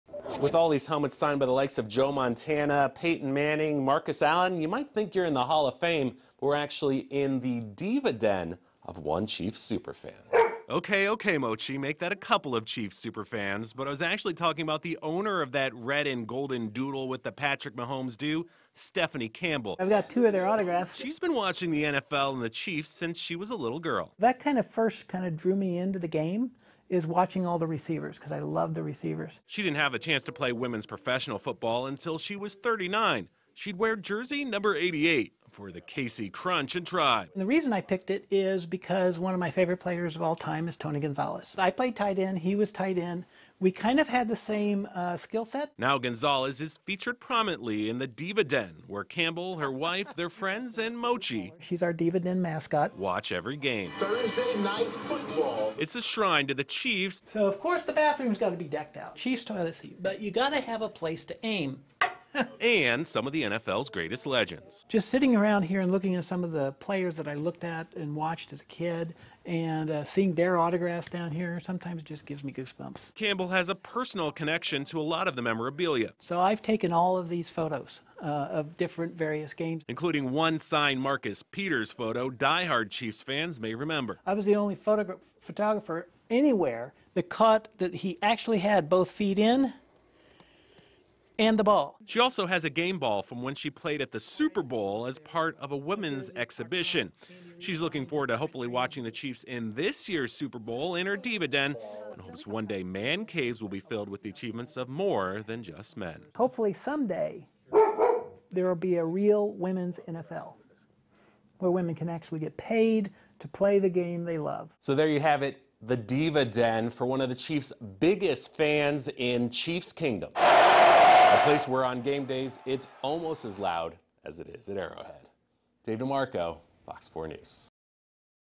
This Diva Den is filled with Chiefs memorabilia
Stare at the wall with signed helmets of all 32 NFL teams, and you might think you`re in the NFL Hall of Fame. But it`s actually the "Diva Den" of one metro couple.